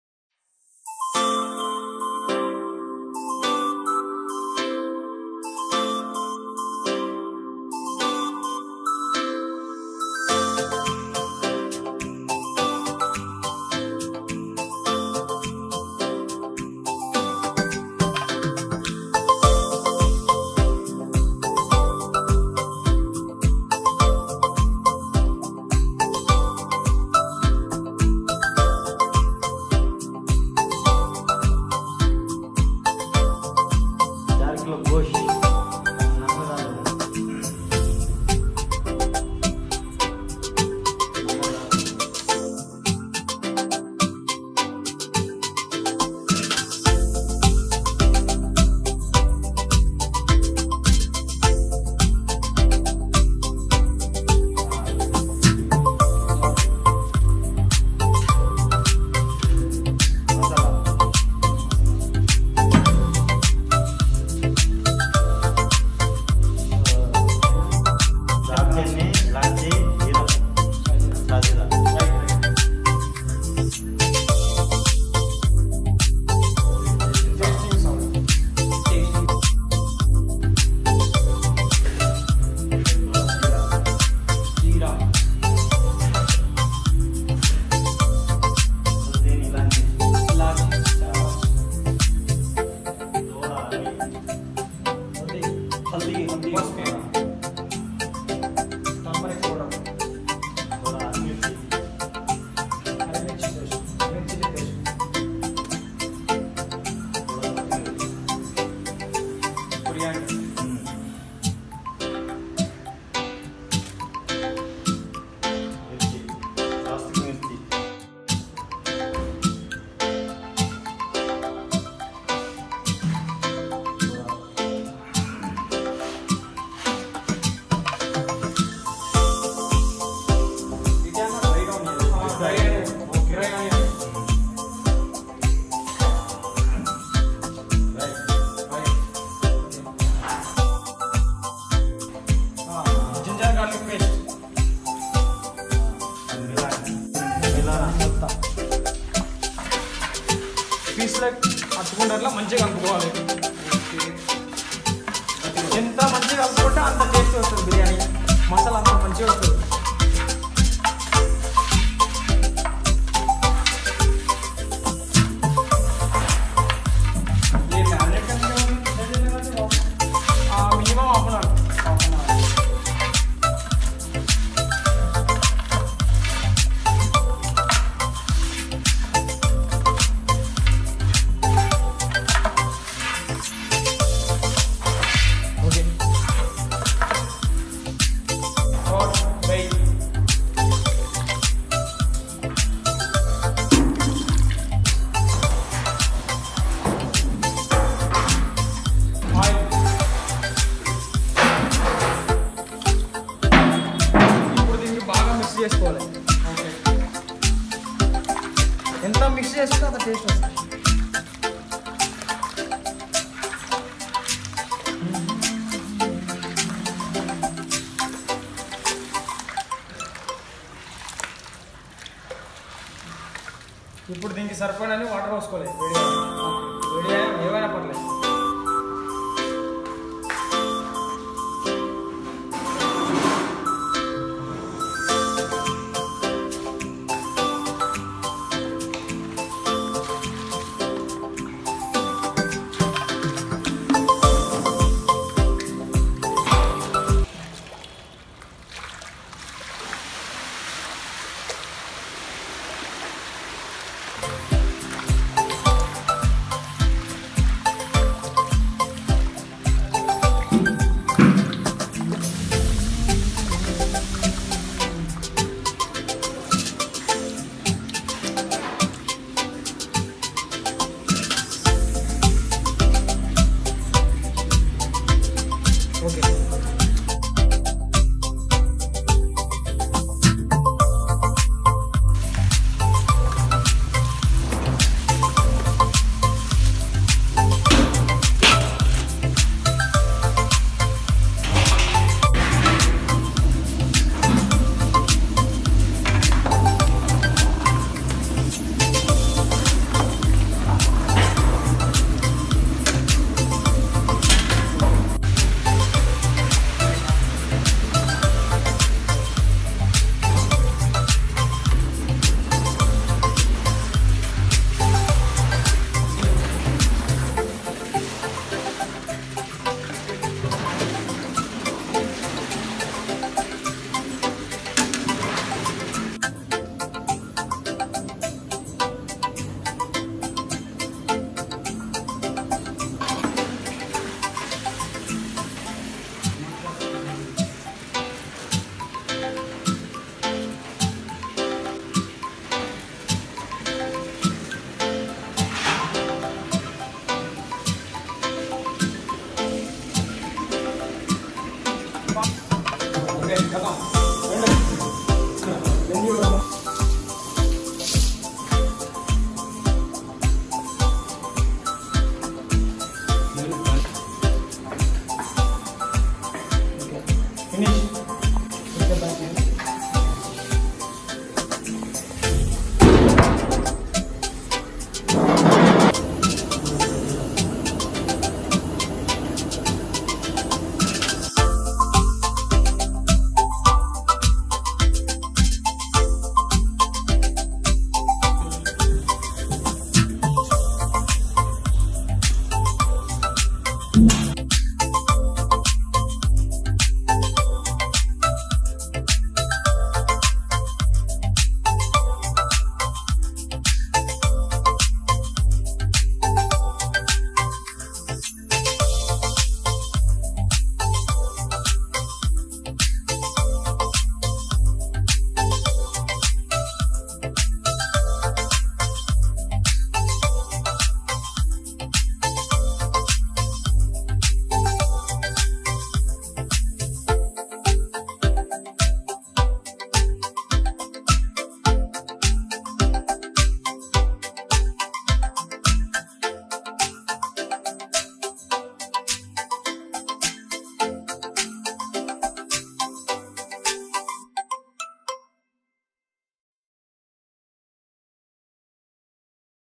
MUTTON BIRYANI | HYDERABADI MUTTON BIRYANI | MASTER CHEF MUTTON BIRYANI
Hello Everyone,

Today we've got a recipe of delicious mutton biryani from hyderabad famous restaurant Master Chef.

Master chef Family restaurant, uppal X road. Opposite to uppal metro station.

Online services available.